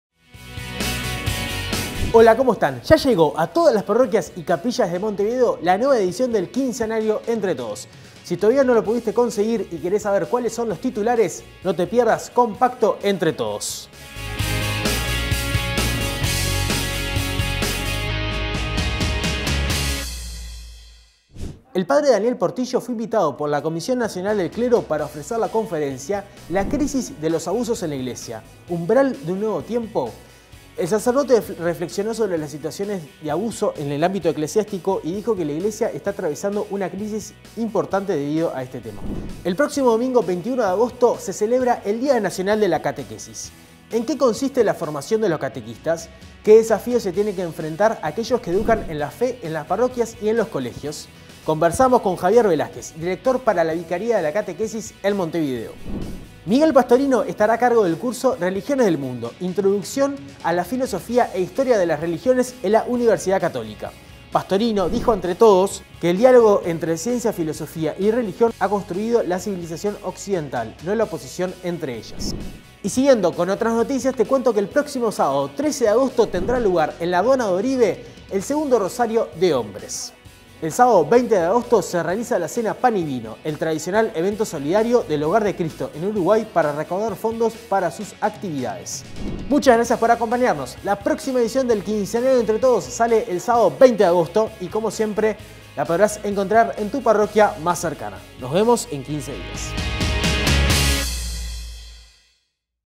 Compacto Entre Todos - Edición N° 521
Ya podés conseguir en tu parroquia el nuevo número del quincenario Entre Todos. Conocé los titulares en CompactoEntreTodos.

Conducción: Fabián "Chapu" Caffa